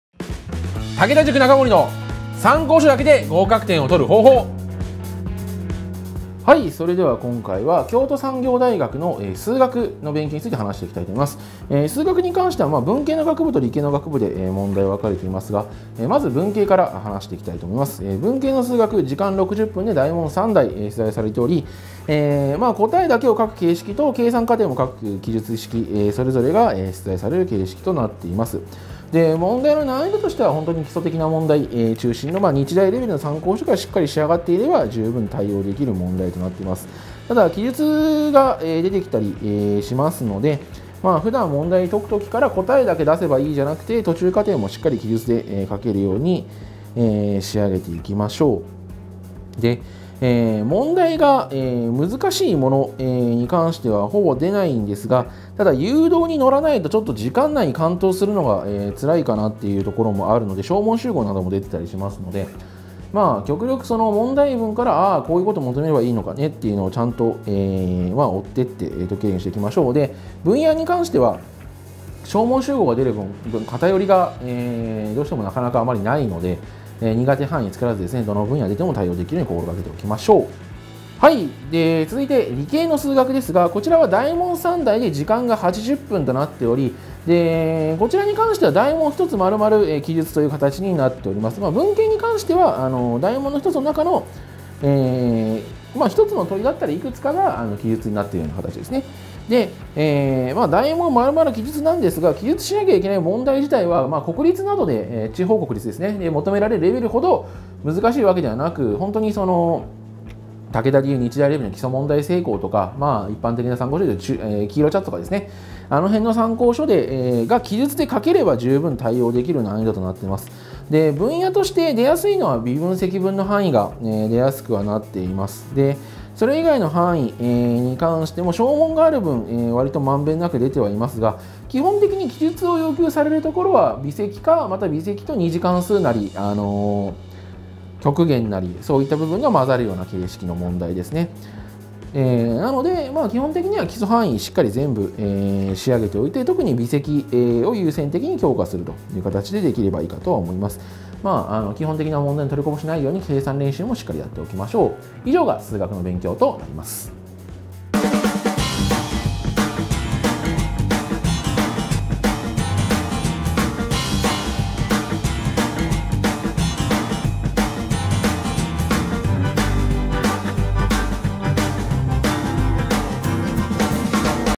参考書だけで京都産業大学ー数学で合格点を取る方法
■参考書だけで京都産業大学ー政治経済・現代社会で合格点を取る方法

日本初！授業をしない。
武田塾